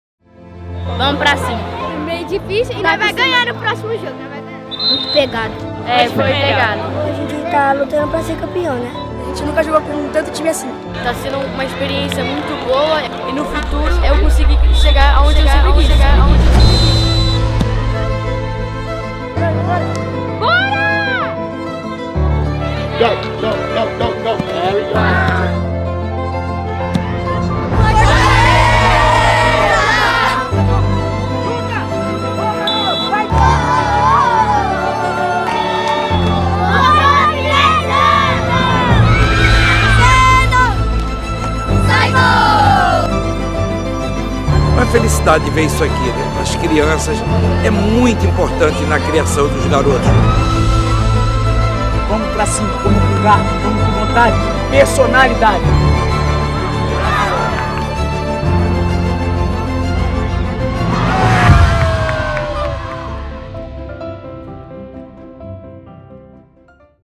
⚽ GOCUP 2024
Muito pegado!!! ⚽🏟️🔥

Esse é o nível do Go Cup e já entramos em clima de reta final da primeira fase, em que qualquer erro pode deixar uma equipe de fora da classificação para seguir disputando o título.🏆

Equipes nacionais e internacionais disputam uma vaga na próxima fase do torneio com muita garra e determinação. 🚀

O terceiro dia foi incrível, tem mais pela frente!